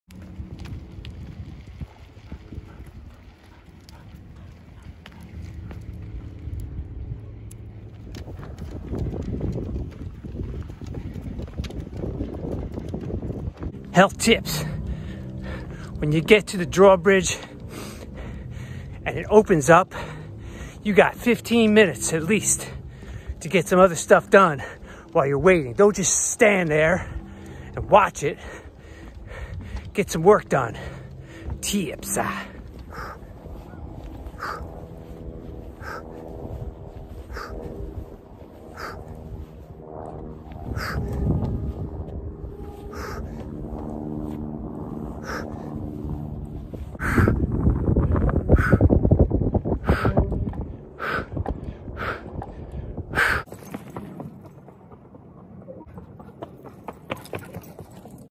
What to do when you get stuck at the bridge.
When out for a walk or run, and you get stuck at the Glimmerglass bridge in Manasquan, instead of just watching it go up and down, get to work!